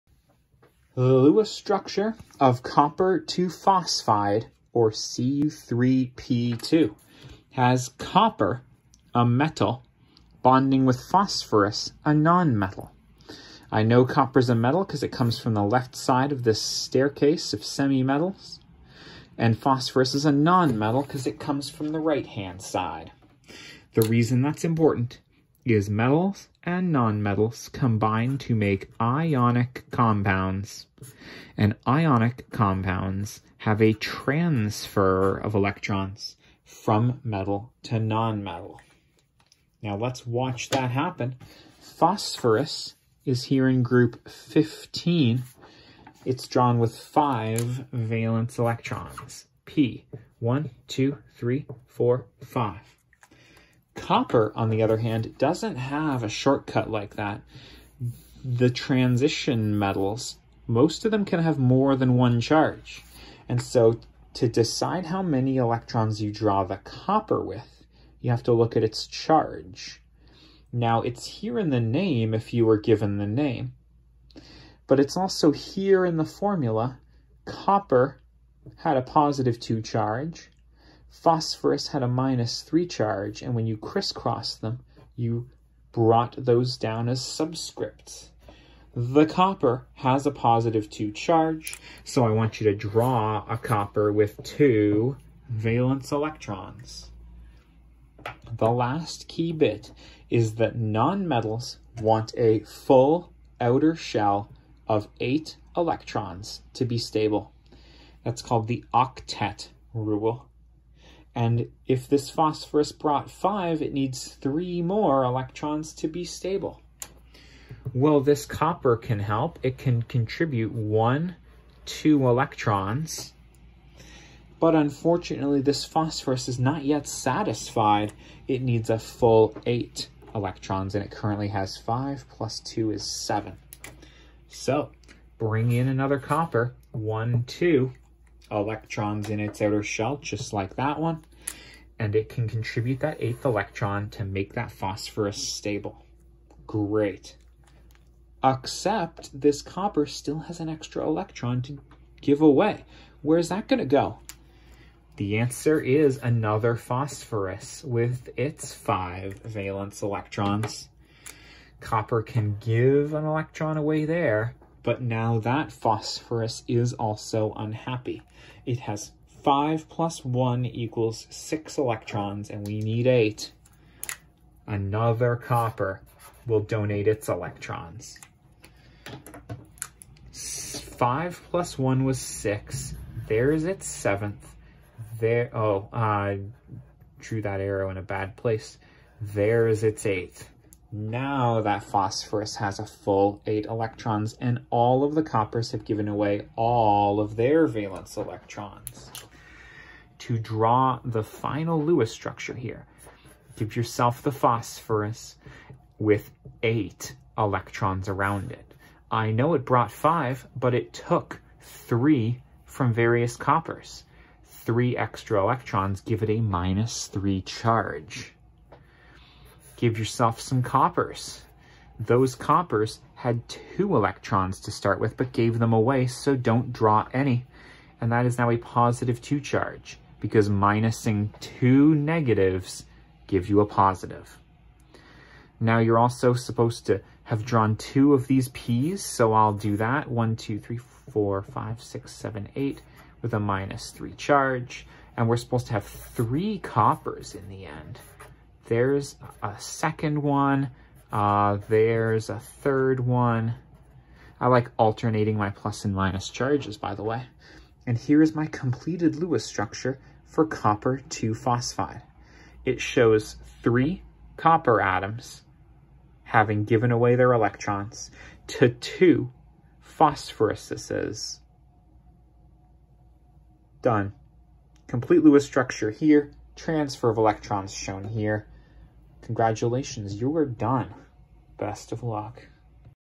Lewis Structure of Cu3P2, copper (II) phosphide
Copper is a metal that can have more than one charge; it this compound it has a charge of +2 so it is drawn with TWO valence electrons to start. It wants to lose these two electrons to become stable.
Phosphorus is a non-metal from Group 15, with FIVE electrons in its outer shell. It wants to gain three electrons to have a full octet (eight electrons) in its outer shell.
So, three copper atoms are needed to donate their six electrons (total) to two phosphorus atoms, and then all five atoms will have become stable.
Cu3P2, copper (II) phosphide, is made and I draw it here for you.